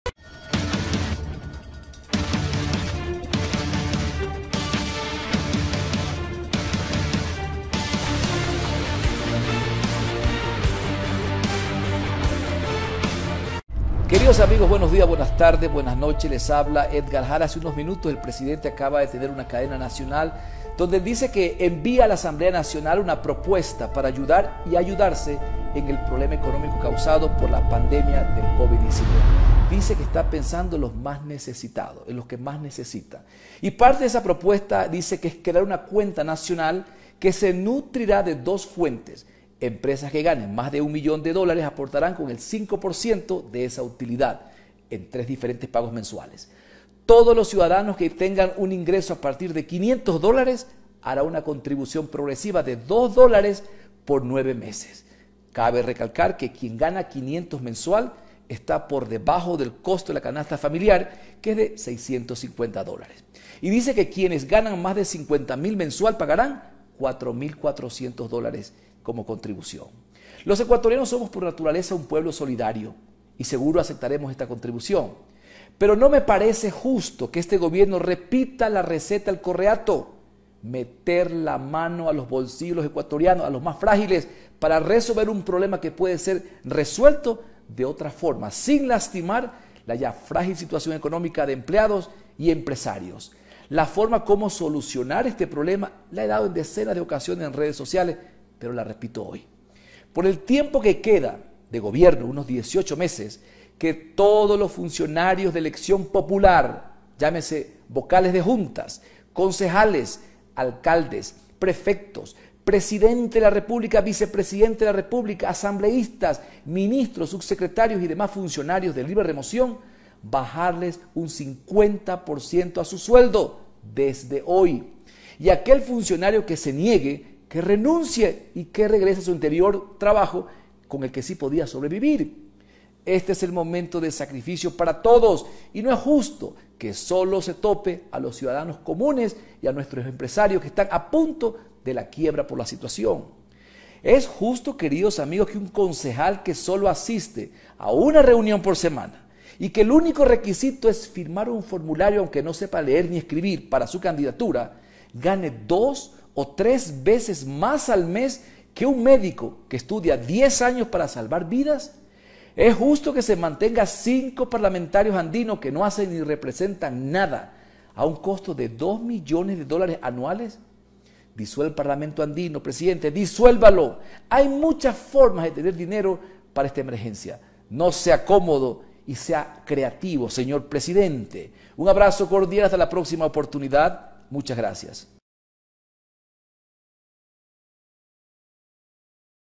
CIUDADANO ECUATORIANO PROPONE MEDIDAS ECONOMICAS A LENIN MORENO
CIUDADANO PROPONE MEDIDAS ECONÓMICAS A  LENIN MORENO